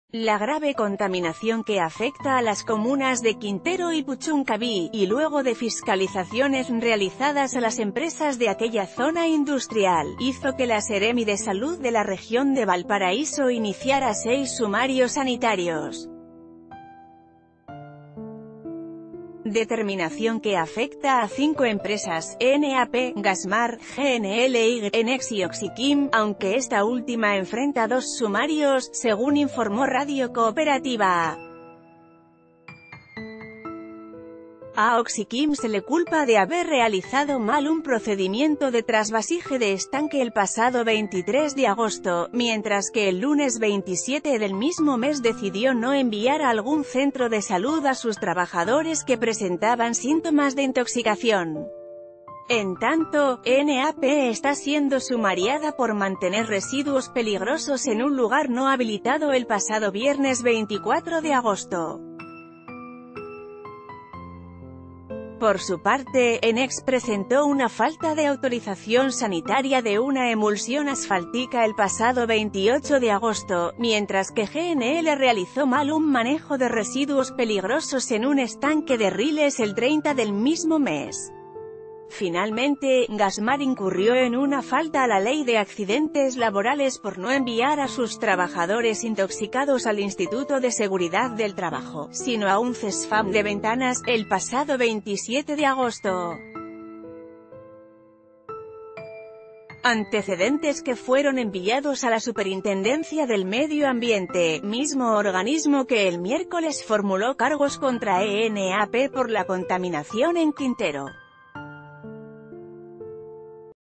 Seremi de Salud abre sumarios sanitarios a cinco empresas en medio de contaminación en Quintero
Cesar Guzman